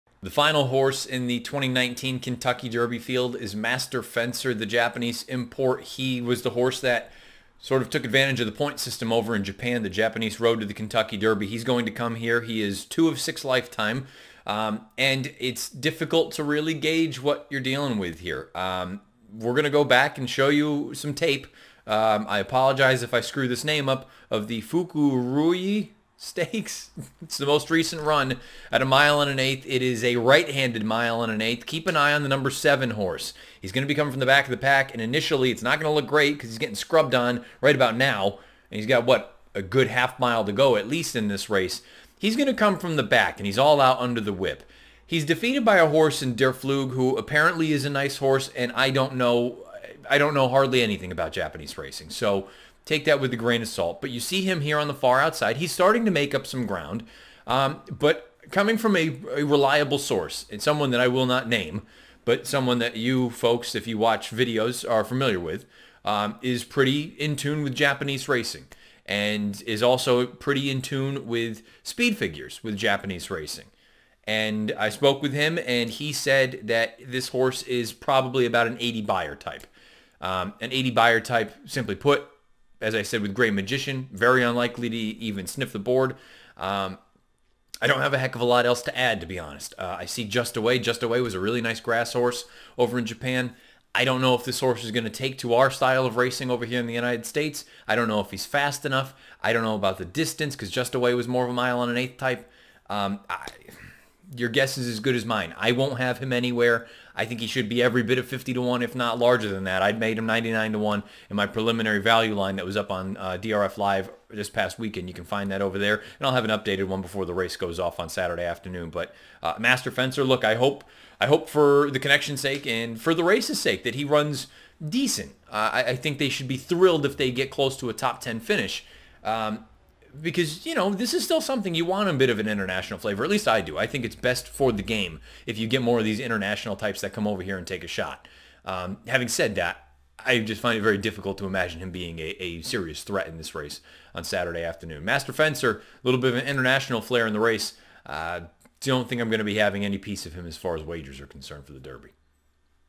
Kentucky Derby Contender Profile - Master Fencer
Matt Bernier takes an in-depth look at Kentucky Derby Contender Master Fencer here on DRFTV